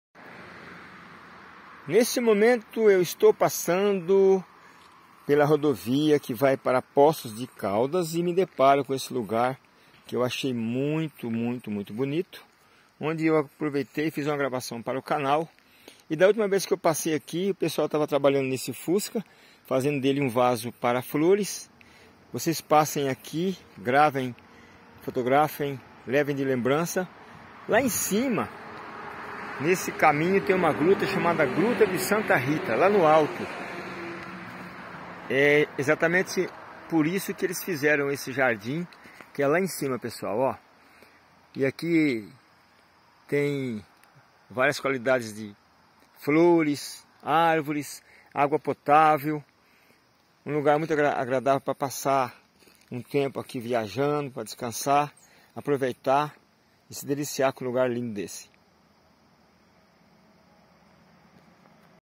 Parada Santa Rita de Caldas
No trevo de Santa Rita de Caldas, em Minas Gerais, próximo de um posto de gasolina há esse lugar simples e bem construído para que os viajantes descansem e bebam uma excelente água de mina, pura e leve. Vale a pena dar uma parada e fazer uma fotos.